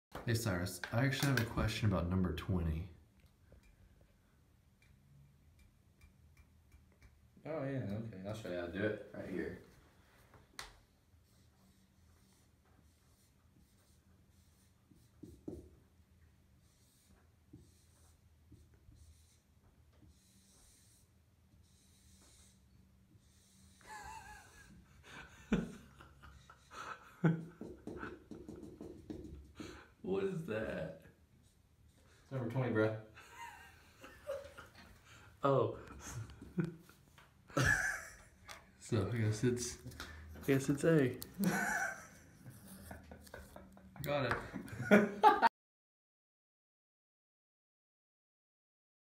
He Helped With My Homework
Every say thank you to Cyrus for helping me with my homework. Thanks, bud. @derekstiltner on twitter